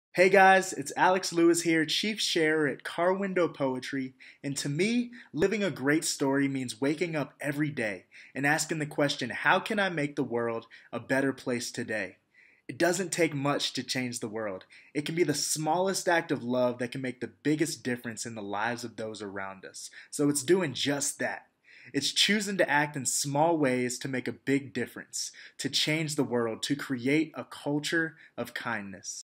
"To me LIVE A GREAT STORY means..." with Alex Lewis of Car Window Poetry
LIVE Ambassador Alex Lewis of Car Window Poetry from Colorado Springs shares what LIVE A GREAT STORY means to him.